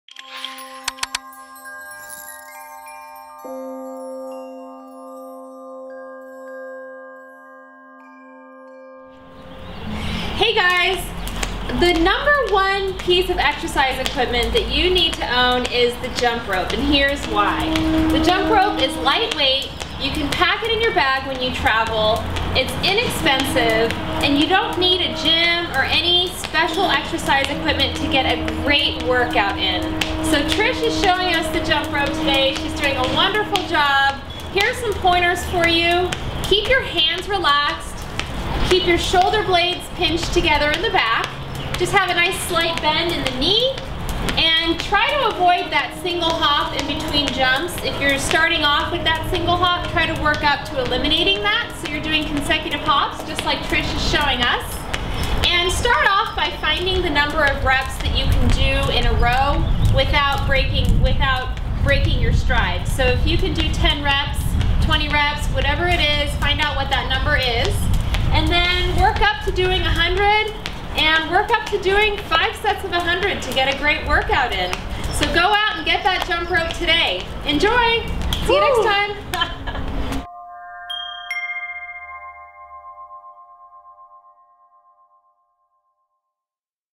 The 1 Piece of Exercise Equipment Everybody Should Own
What is the one piece of exercise equipment everybody should own? A jump rope! It’s lightweight, inexpensive, easy to store, and you can take it along when traveling to get a killer workout.

That said, sometimes it's not as easy to pick up the jump rope as it was when you were a kid. But don't worry, it's like riding a bike — it will come back to you. To help you get going, here are some tips and tricks.